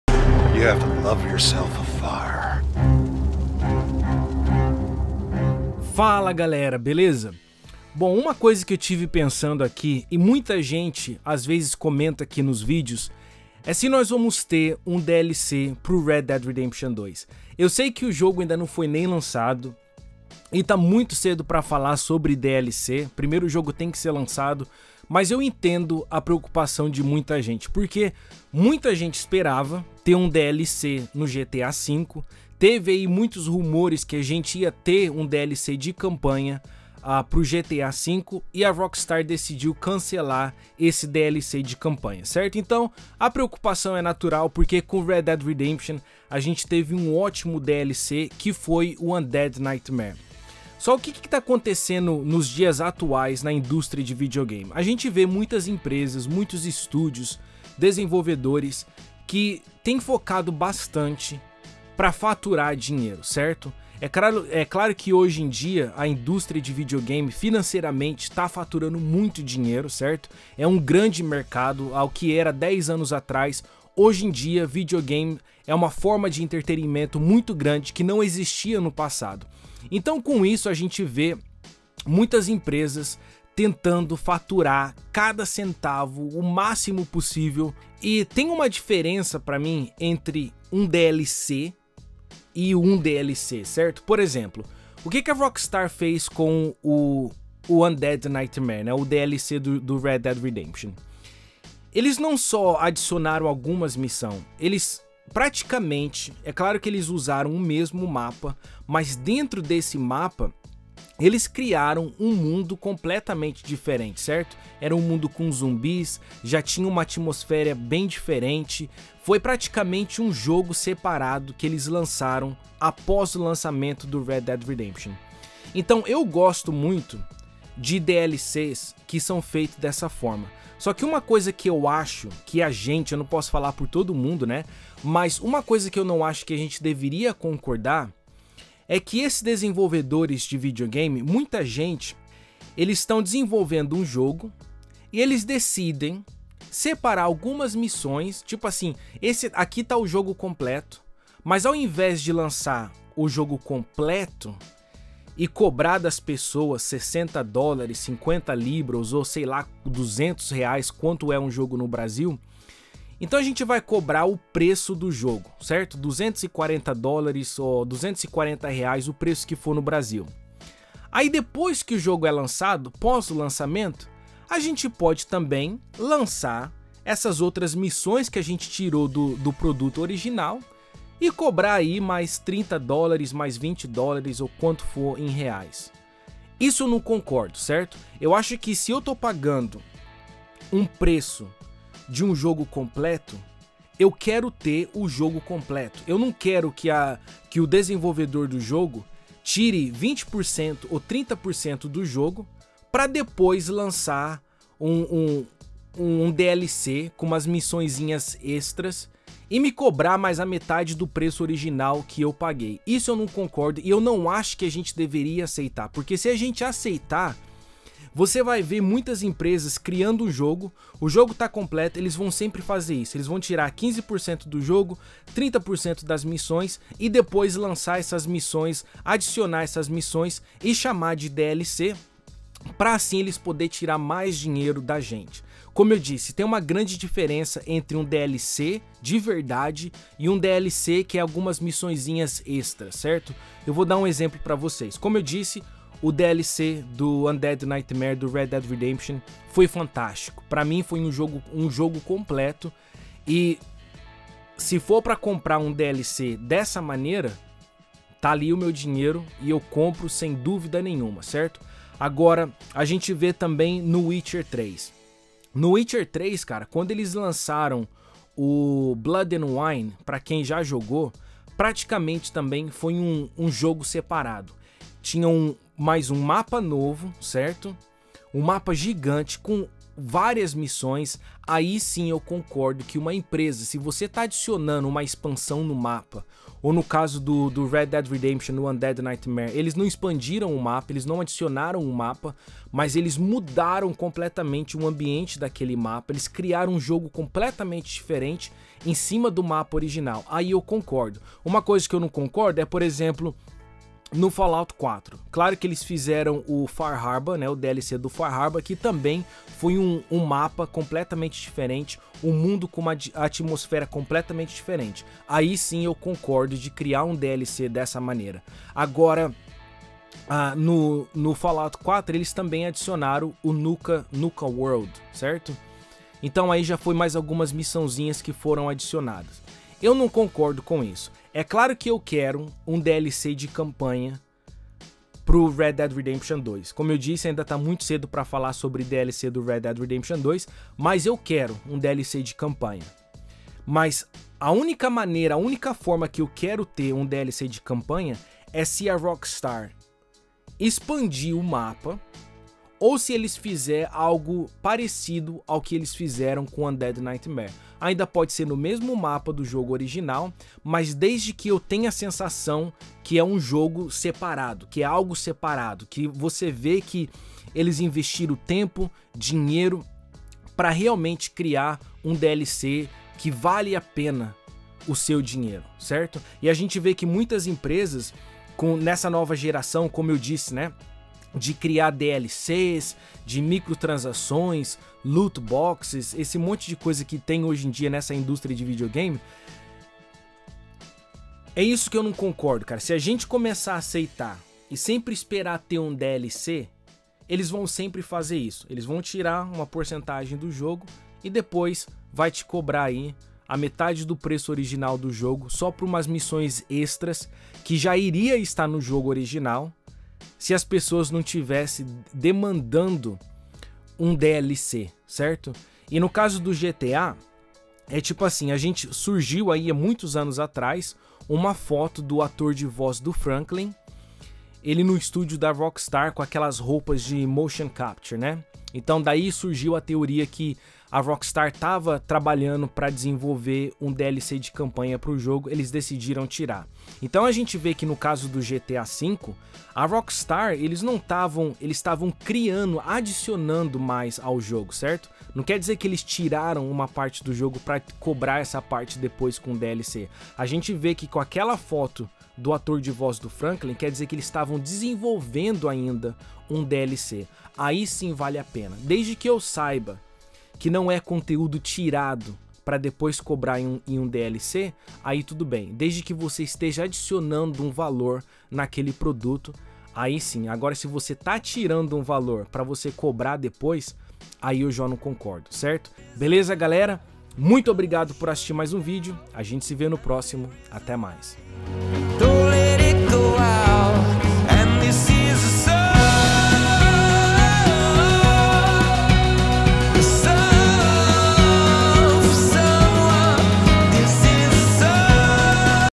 Red Dead Redemption 2 : Você Quer um DLC??
A rockstar criou ótimos DLCs no passado e depois do sucesso do DLC do Undead Nightmare muitos acreditaram que a Rockstar também iria lançar um DLC de campanha para o GTA V.
Neste videos nós vamos descultir se realmente vale a pena fazer um DLC para o Red Dead Redemption 2